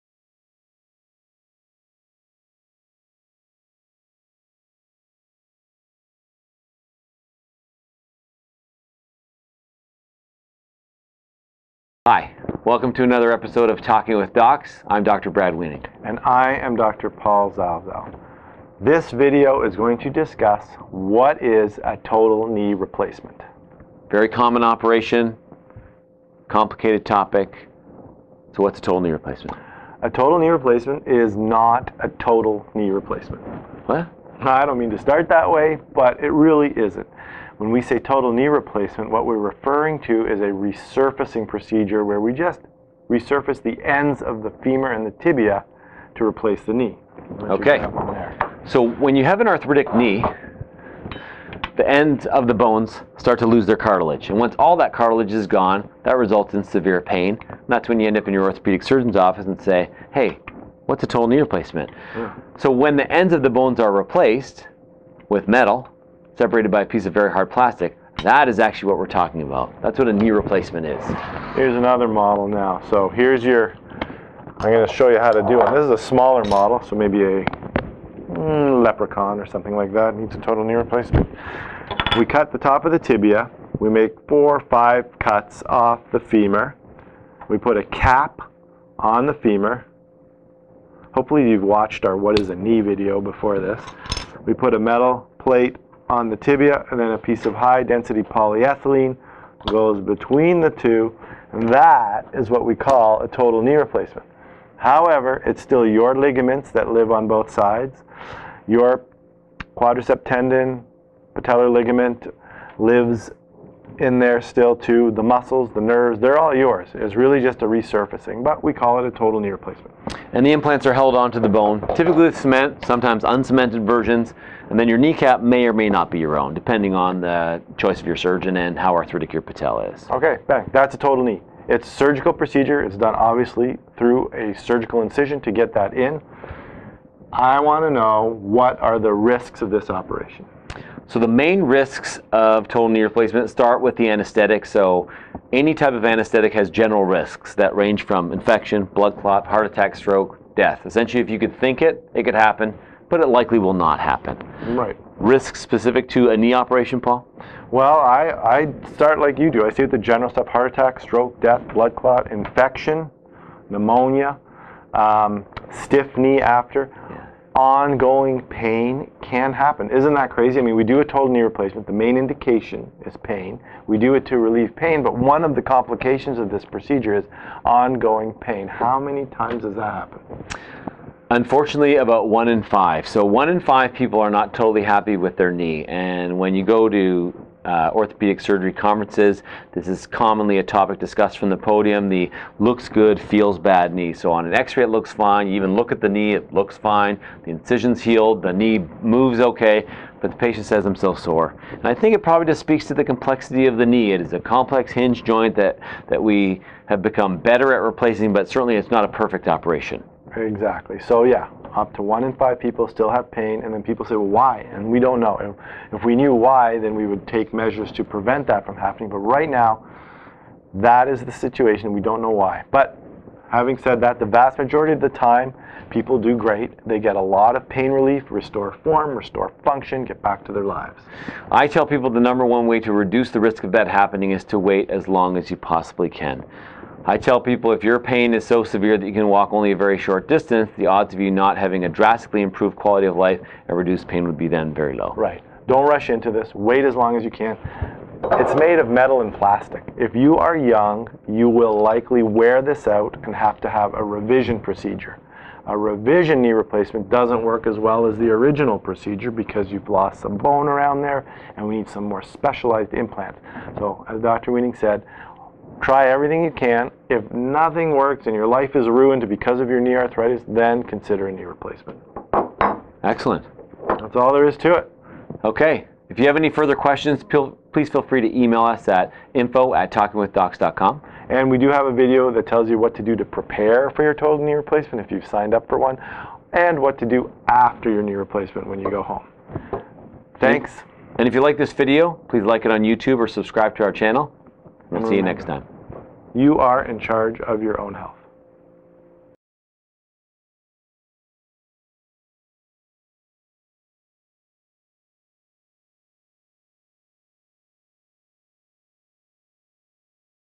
Everything You Ever Wanted To Know About Total Knee Replacement Surgery
This video discusses total knee replacement. The procedure and some of its risks are discussed.. Although total knee replacement is a very safe procedure, risks  include heart attack, stroke, death, blood clot, infection, ongoing pain, nerve injuries, vascular injuries, fractures, and others.  Total knee replacements can make clicking or clunking sounds when implanted.